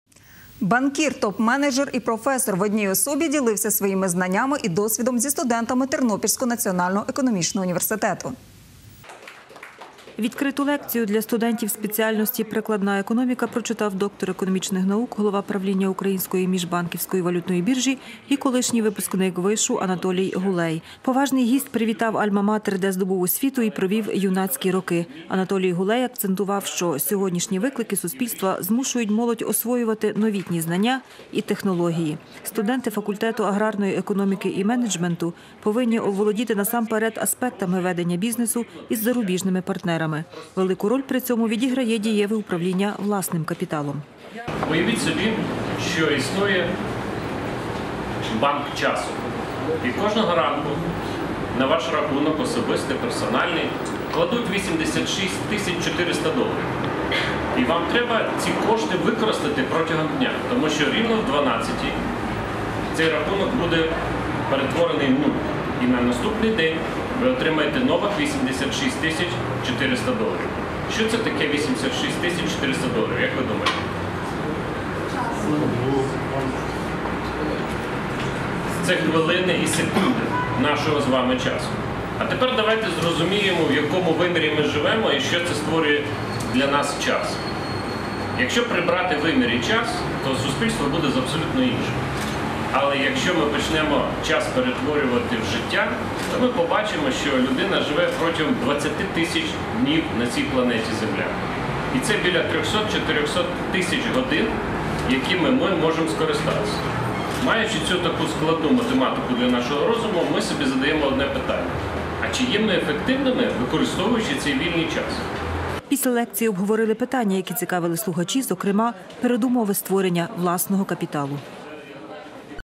Про управління власним капіталом дізналися студенти ТНЕУ
Для студентів ТНЕУ та усіх бажаючих відбулася лекція на тему «Управління власним капіталом», яку прочитав доктор економічних наук, голова Правління Української міжбанківської валютної біржі Анатолій Гулей